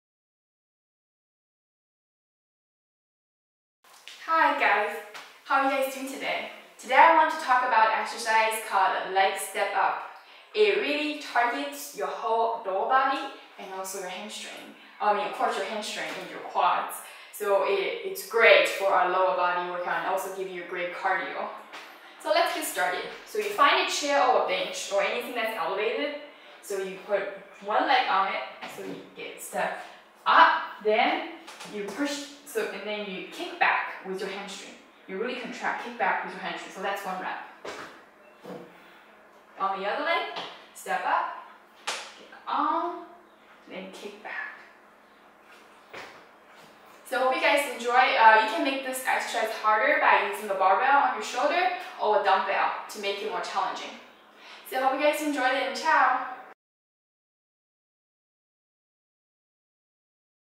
How to perform Stepup with Kickback
This video shows you how to perform Stepup with Kickback including advanced modifications.